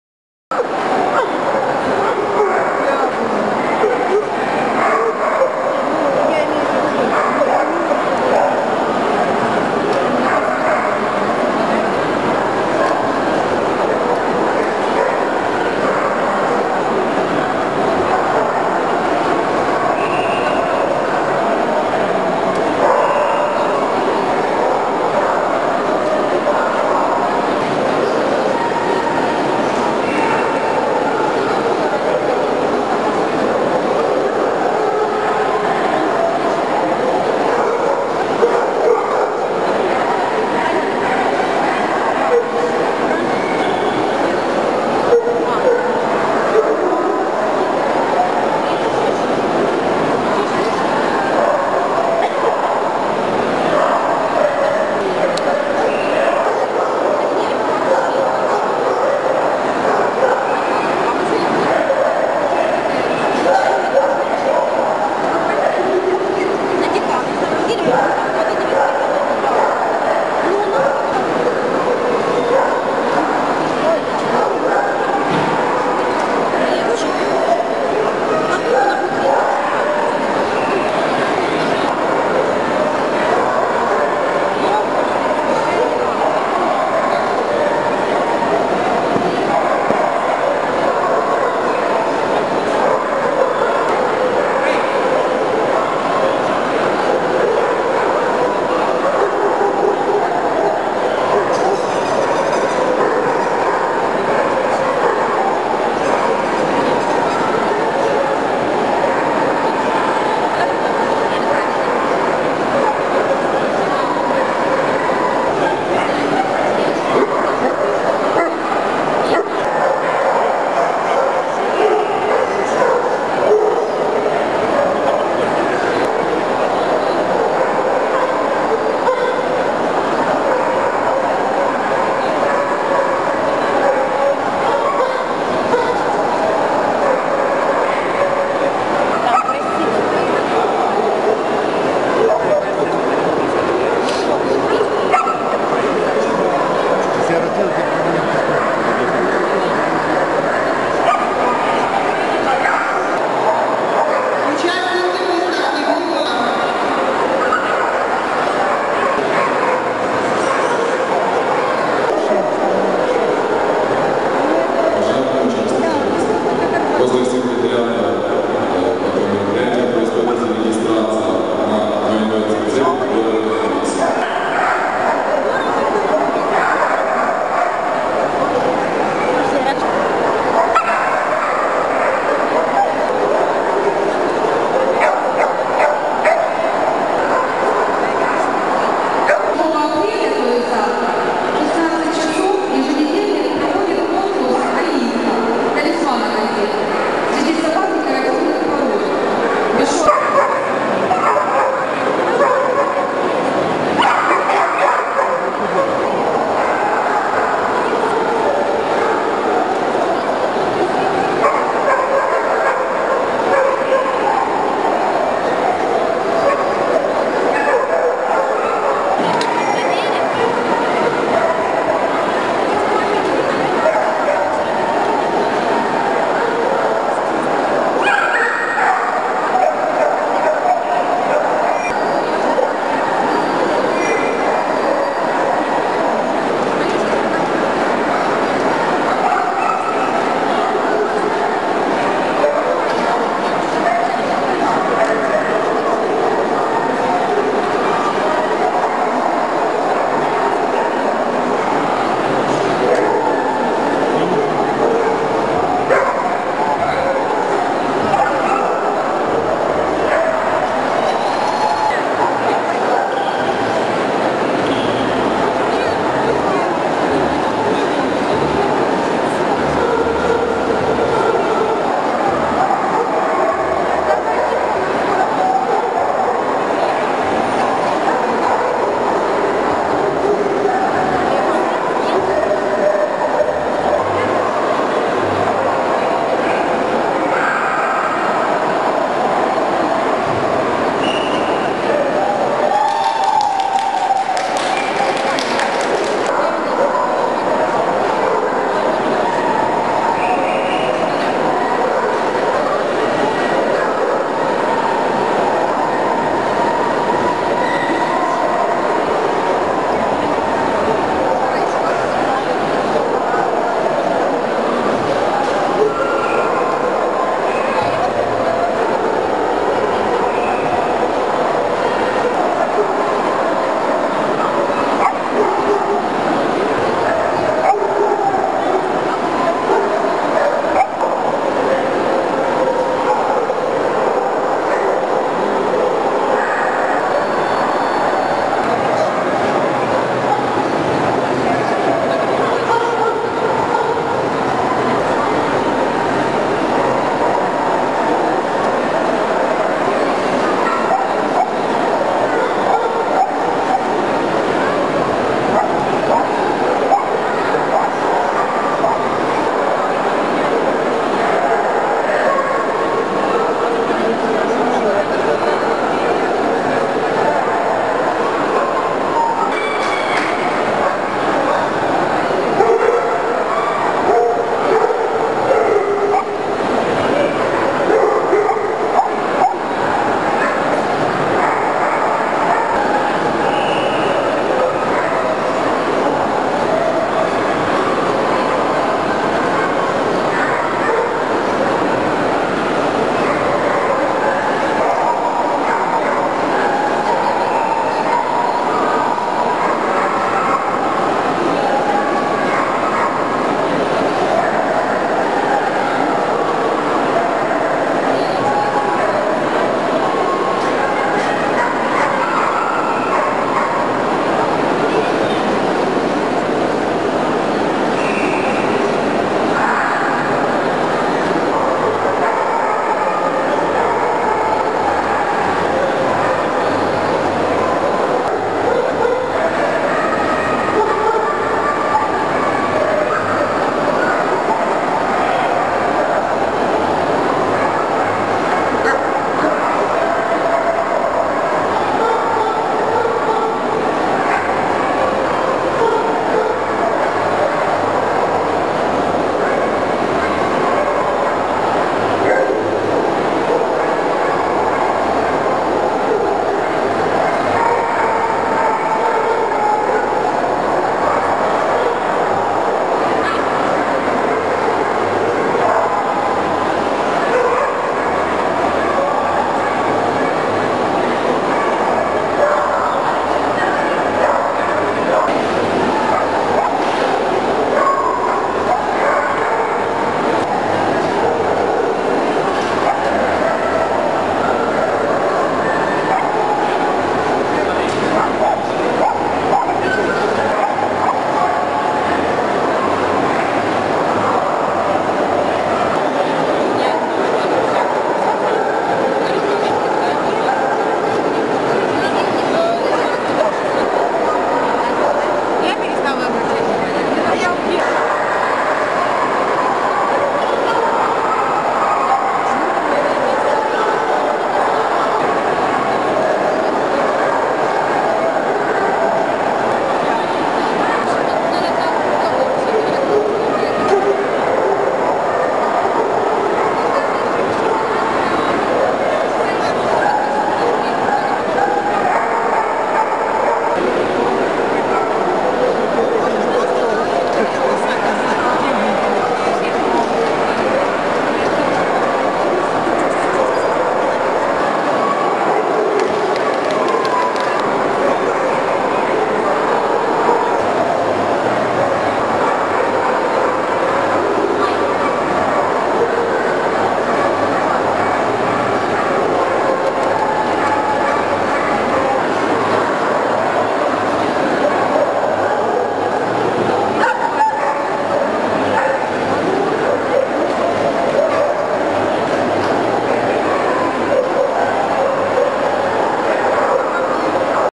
Dog Show Ukraine, Kyiv - apr 2008, p.1
Dog Show Ukraine, Kyiv - apr 2008   St.Bernard, beagle, malemute, newfoundland